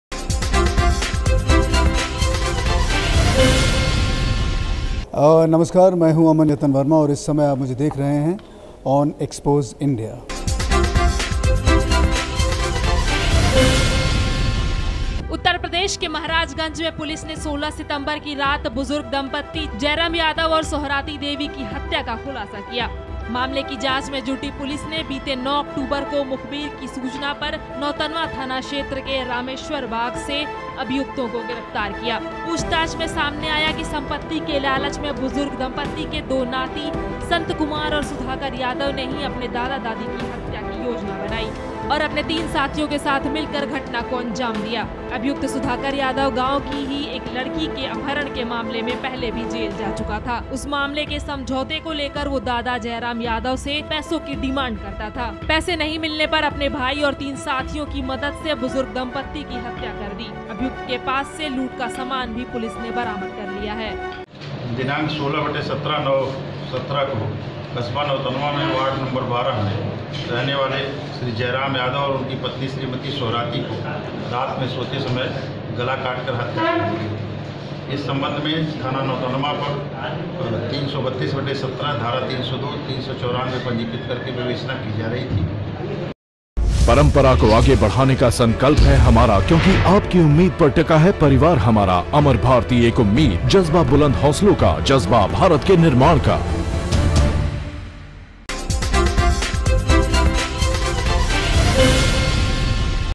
खुलासा: बुजुर्ग दंपति की हत्या मिटाये सबूरत लेकिन पुलिस की चतुराई से खुल गया राज़
महाराजगंज जिले में नौतनवा थानाक्षेत्र के क़स्बे में बीते 16 सितंबर की रात बुजुर्ग दंपति जयराम यादव और सोहराती देवी की गला काटकर हत्या और ढाई लाख रुपये के साथ सोने चाँदी के जेवर की लूट हुई थी। इस जघन्य हत्या से एक तरफ जहां क्षेत्र में दहशत फैल गई थी वहीं  महराजगंज पुलिस के लिए कड़ी चुनौती भी बनी थी हालांकि महाराजगंज के पुलिस अधीक्षक आर पी सिंह ने इस सनसनीखेज वारदात के खुलासे के लिए, जिले के स्वाट टीम प्रभारी दिलीप कुमार और एस.ओ. नौतनवा प्रहलाद पाण्डे की संयुक्त टीम गठित कर दिया था और संपत्ति को इस हत्या का आधार बनाकर पुलिस जांच में जुट गई।
24 दिनों तक लगातार इस जघन्य मामले की जांच में जुटी महराजगंज पुलिस ने बीते नौ अक्टूबर को मुखबिर की सूचना पर नौतनवा थाना क्षेत्र के रामेश्वर बाग से अभियुक्तों की गिरफ्तारी कर ली। पूछताछ में सामने आया कि बुजुर्ग दंपति के दो पौत्रों सन्त कुमार और सुधाकर यादव ने साजिश के तहत मिलकर संपत्ति के लालच में अपने ही दादा-दादी के कत्ल की योजना बना डाली और बीते 16 सितंबर की रात को अपने तीन अन्य साथियों के साथ मिलकर घटना को अंजाम दे दिया। पकड़े गए बुजुर्ग दंपत्ति के पौत्रों संत कुमार और सुधाकर यादव की निशानदेही पर उनके अन्य 3 साथियों को भी पुलिस ने गिरफ्तार कर लिया है।